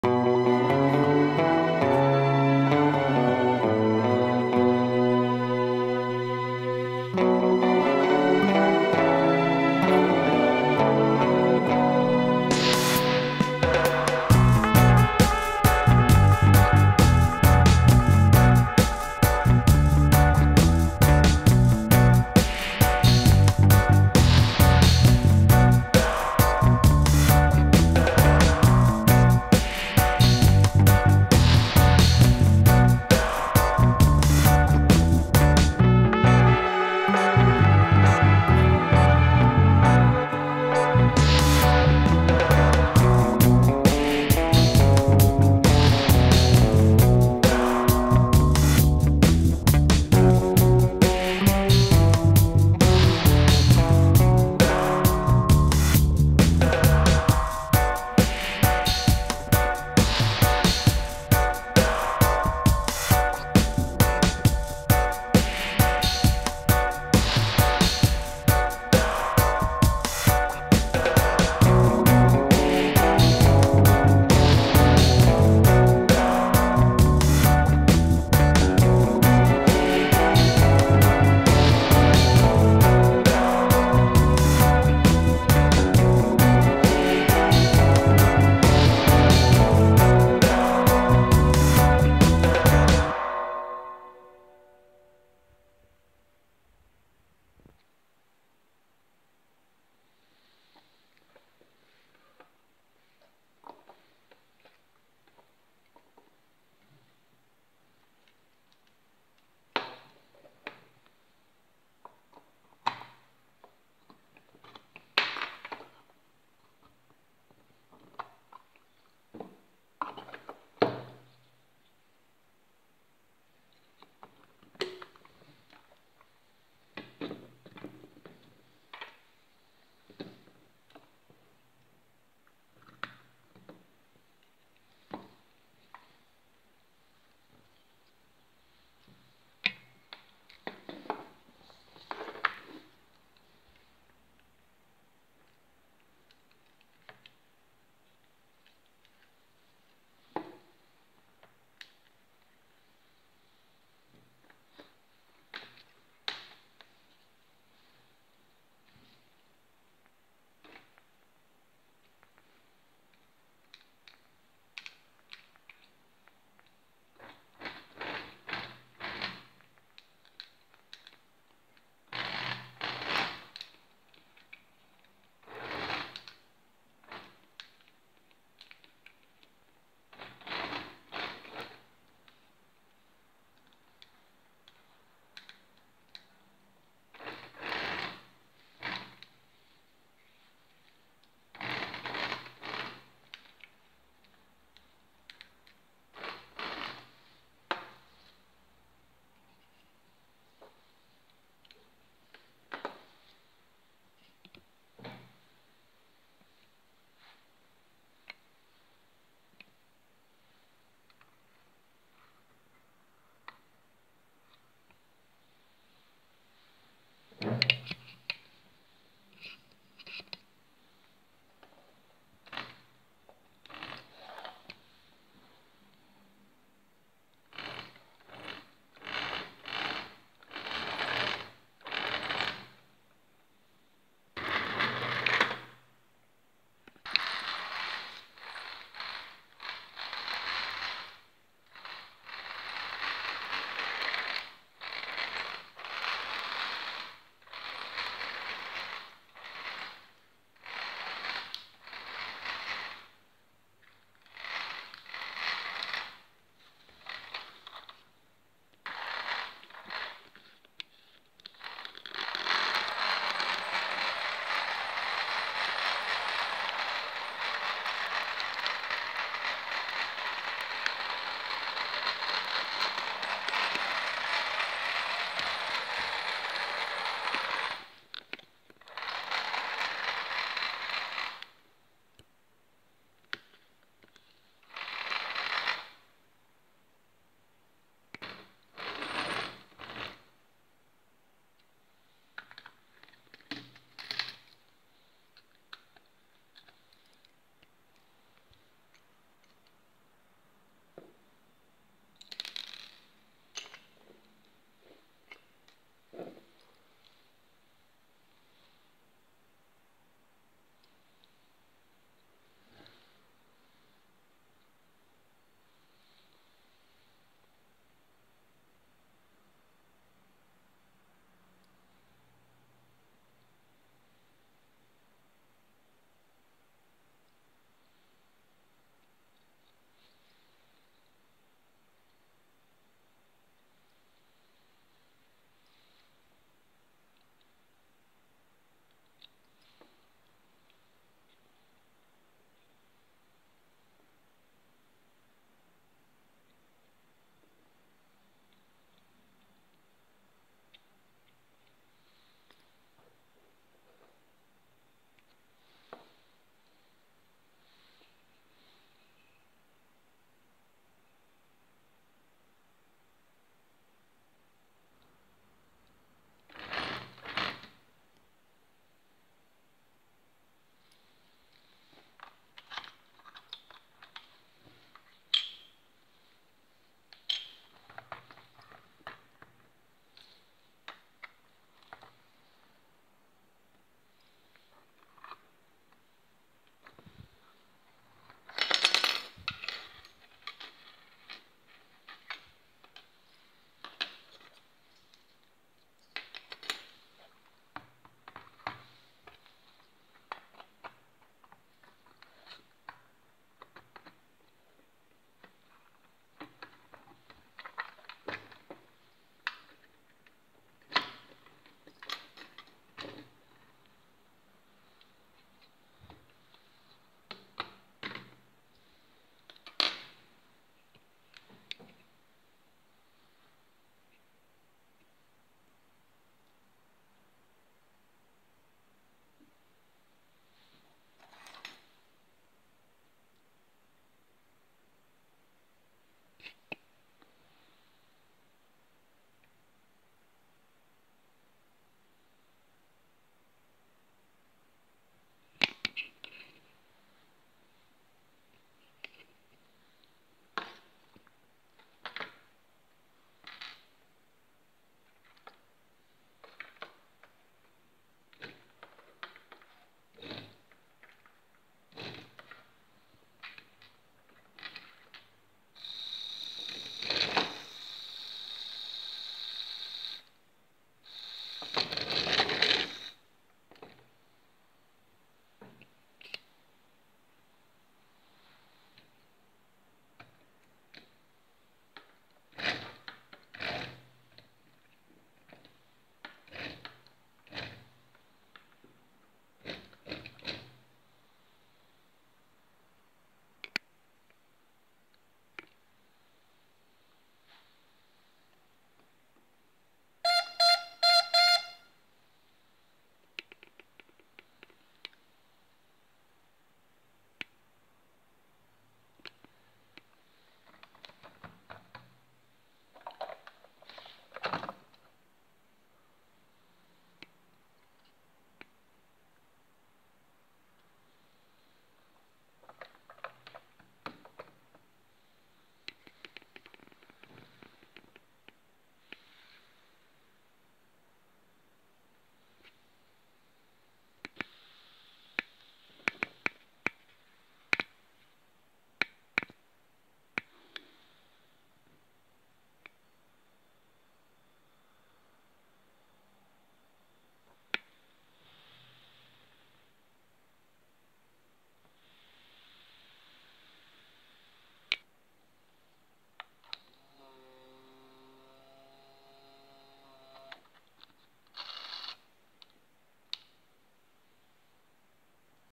1962 Antique Toy Car Presu DDR Wartburg 311 Cabrio Battery operated / stare zabawki PRL Pewex
Wartburg 311 ( Presu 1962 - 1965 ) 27DM